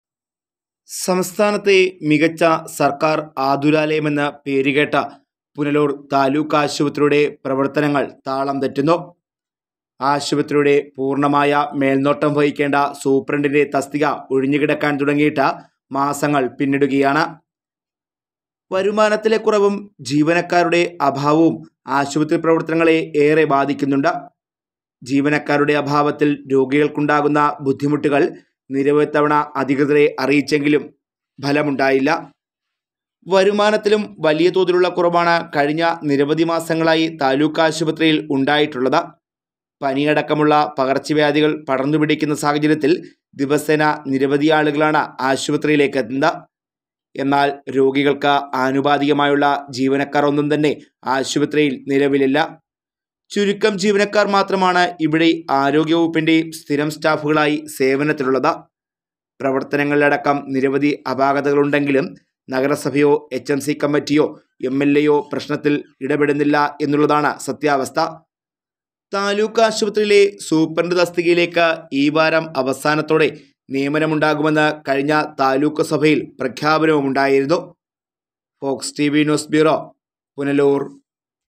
സംസ്ഥാനത്തെ മികച്ച ആതുരാലയമെന്ന് പേരെടുത്ത പുനലൂർ താലൂക്ക് ആശുപത്രിയുടെ പ്രവർത്തനങ്ങൾ താളംതെറ്റ
സംസ്ഥാനത്തെ മികച്ച ആതുരാലയമെന്ന് പേരെടുത്ത പുനലൂർ താലൂക്ക് ആശുപത്രിയുടെ പ്രവർത്തനങ്ങൾ താളംതെറ്റുന്നു .